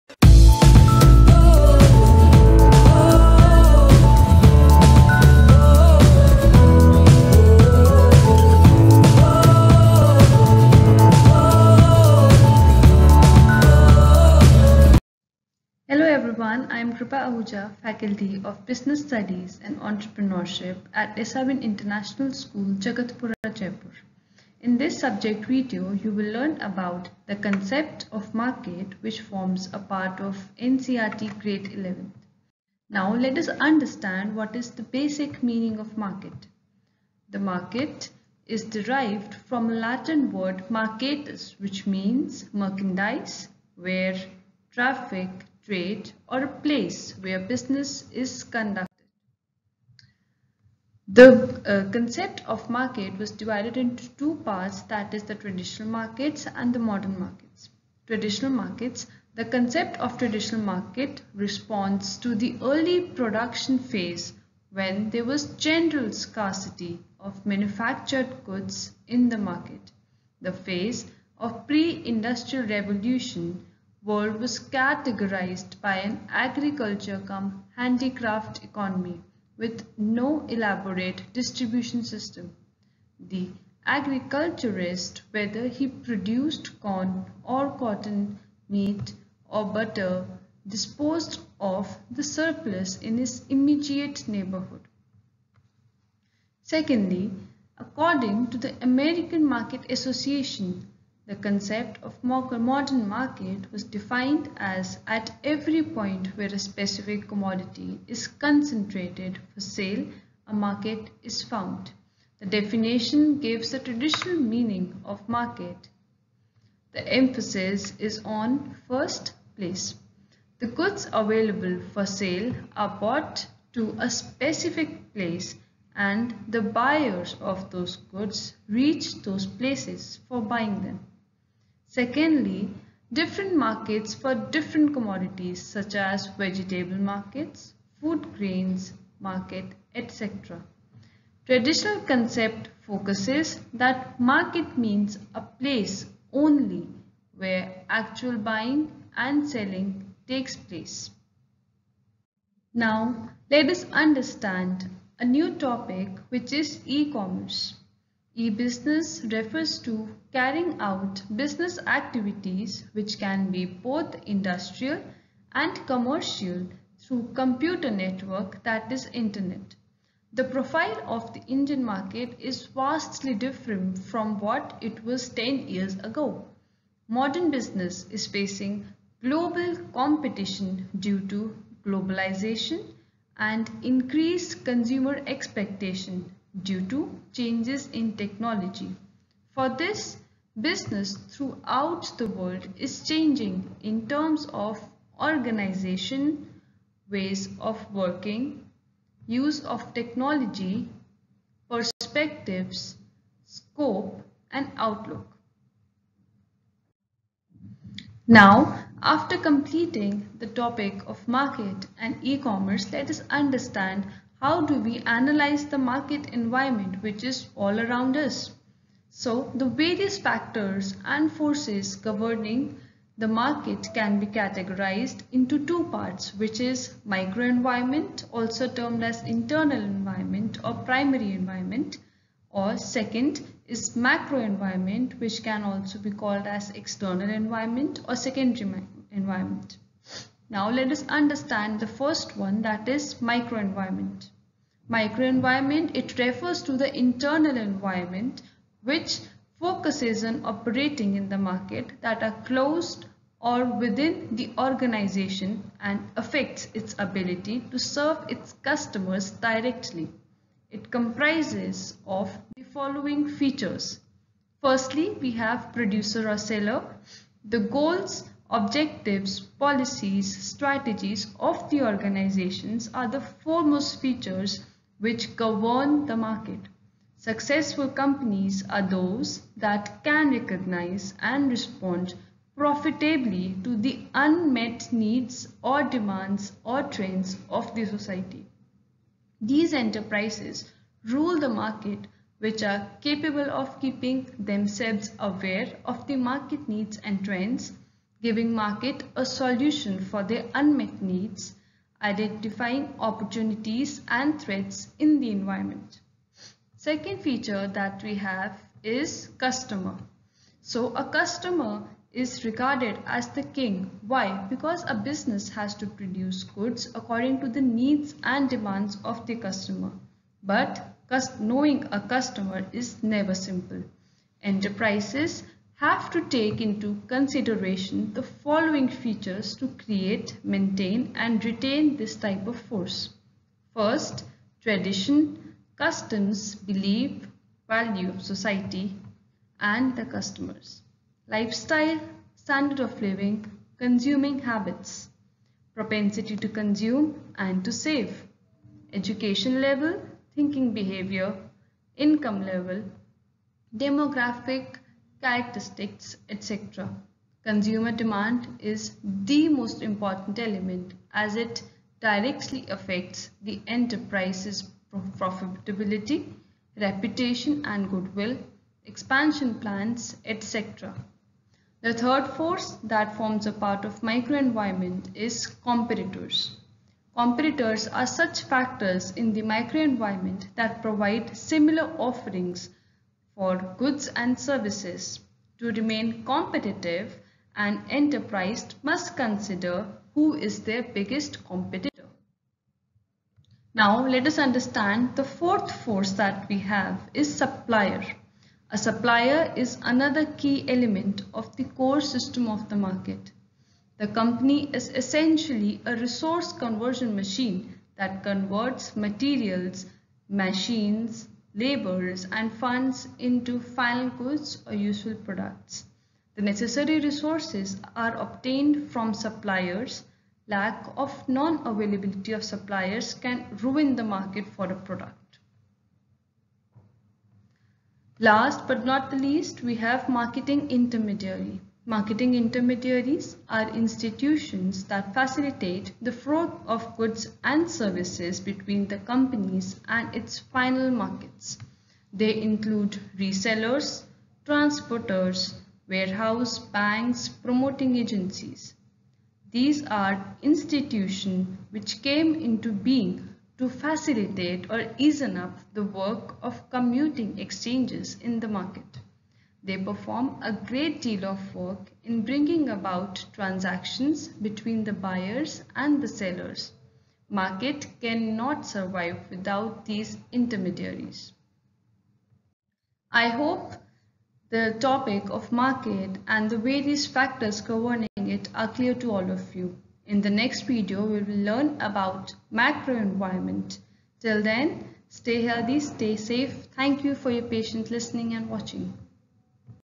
Concept of Market|Grade: XI|Entrepreneurship|SRN Subject Video -159|SRN International School,Jaipur
SRN Subject Video-159 for the students of Grade: XI on the topic : Concept of Market by Ms Kripa Ahuja
Faculty of Entrepreneurship,SRN International School, Jaipur, Rajasthan.